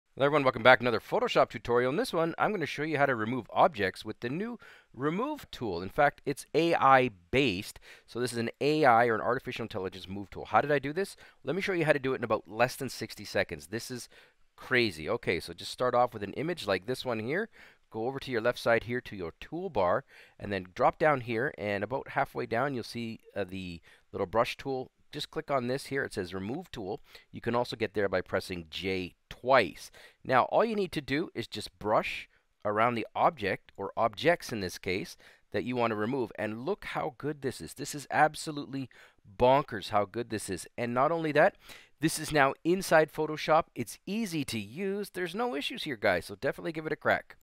Photoshop 2023 (AI) - Ultimate Eraser: Delete Anything In Under 45 Seconds With New Remove Tool
In this quick video I show you how to remove anything from an image or photo using the new AI "Remove tool" in Photoshop 2023.  Delete objects, subjects, people, from a photo with one click.